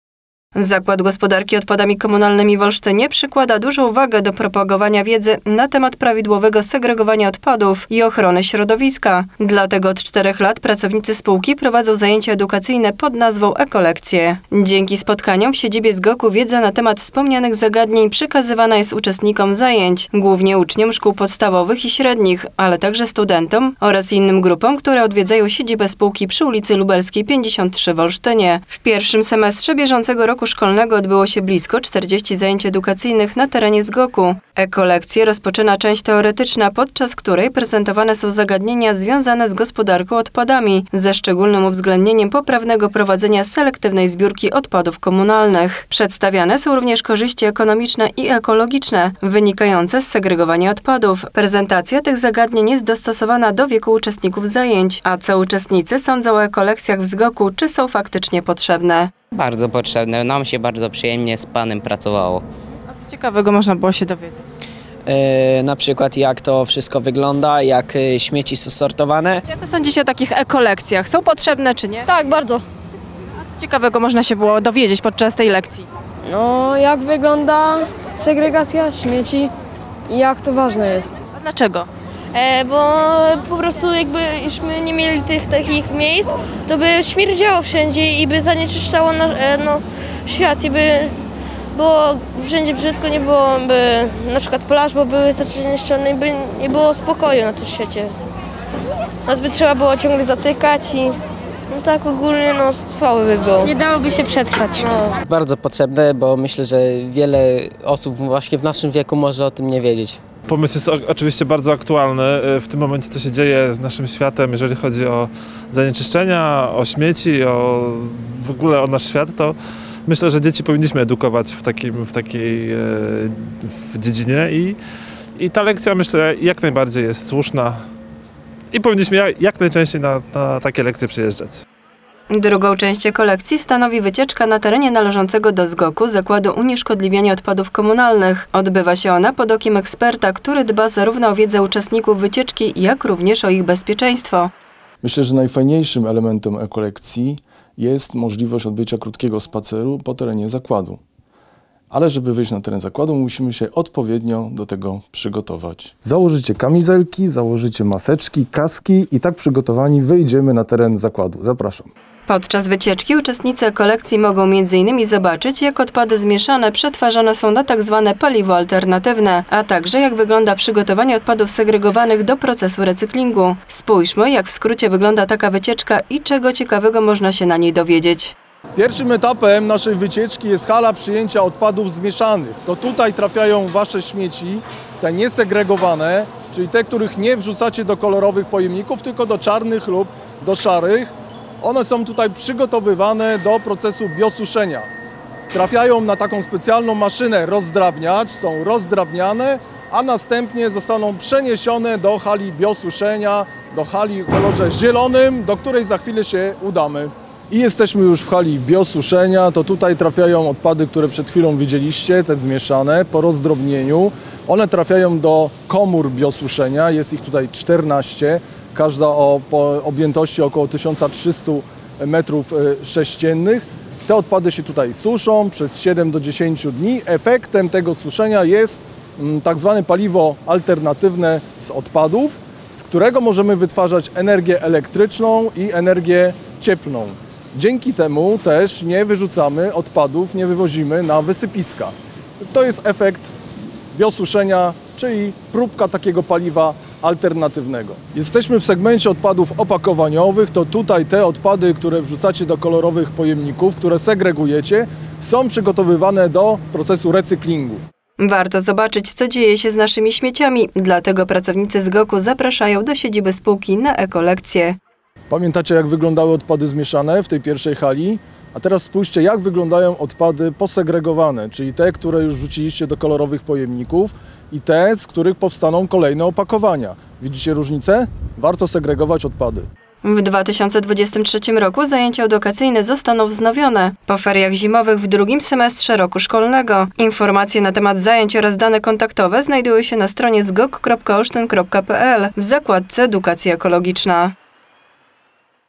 40 zajęć edukacyjnych na terenie ZGOK-u
Zakład Gospodarki Odpadami Komunalnymi w Olsztynie przykłada dużą wagę do propagowania wiedzy na temat prawidłowego segregowania odpadów i ochrony środowiska, dlatego od czterech lat pracownicy spółki prowadzą zajęcia edukacyjne pod nazwą Ekolekcje.